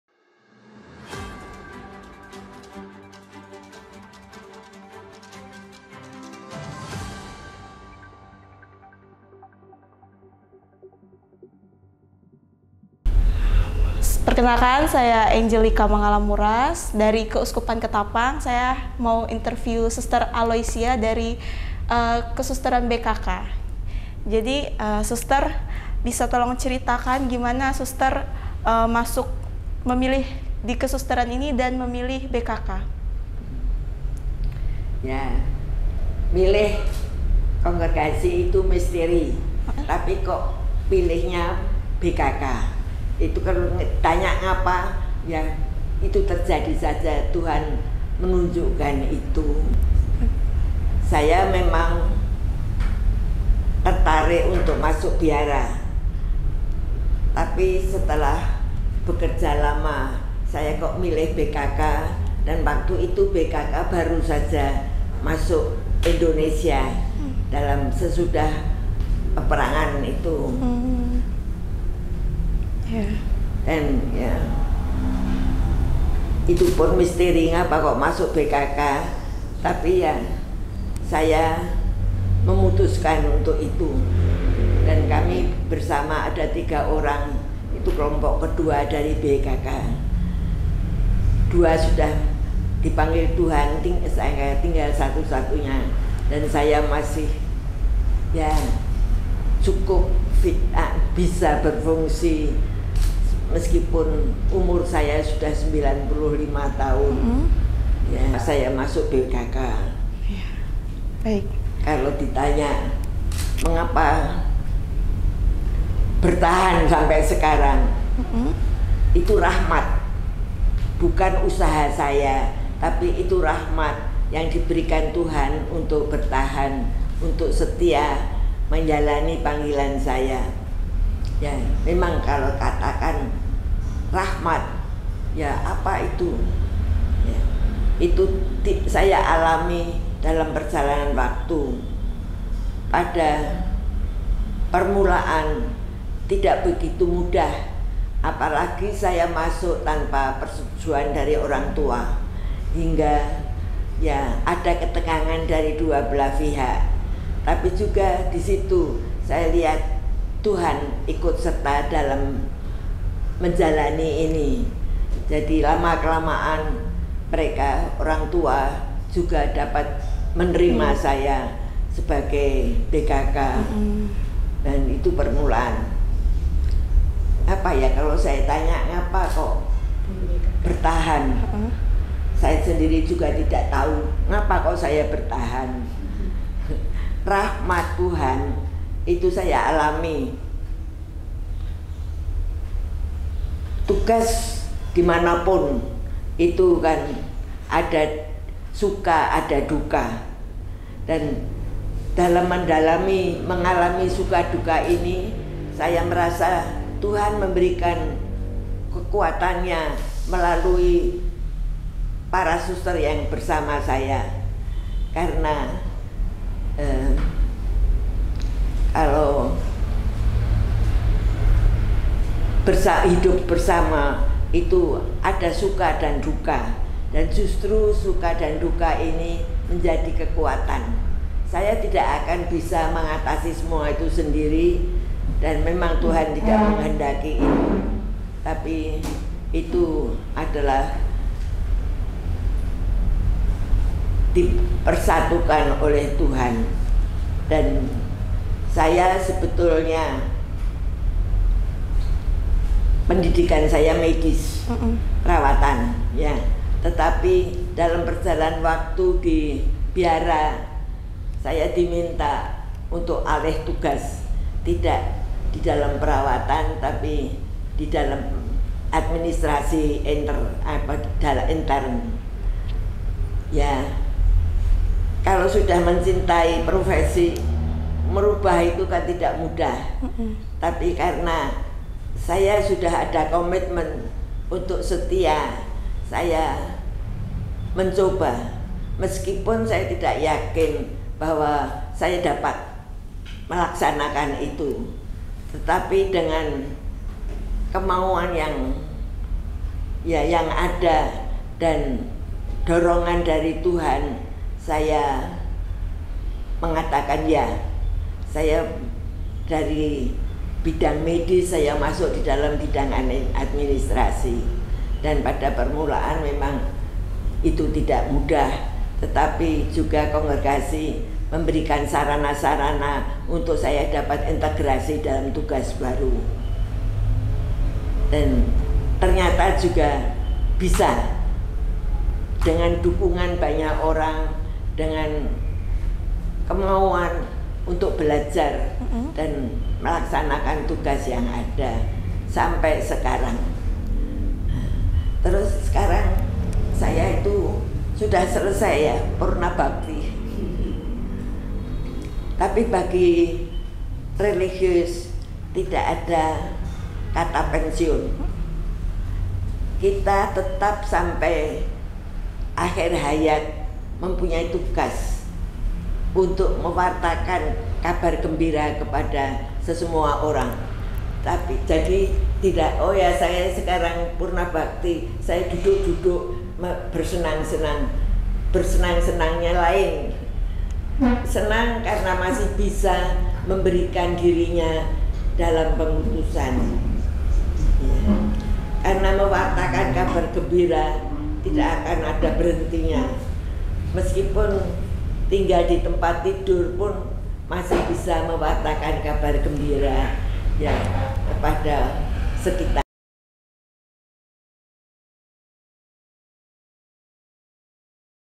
Sr. Aloysia BKK (95): Religius tak Kenal Pensiun
Sr. Aloysia BKK di tahun 2023 ini berusia 95 tahun. Namun ia masih canthas bicara; memori juga masih kuat.

Ia merupakan suster pribumi pertama dan provinsial pribumi pertama Biarawati Karya Kesehatan atau BKK.

Kredit: Titch TV/Mathias Hariyadi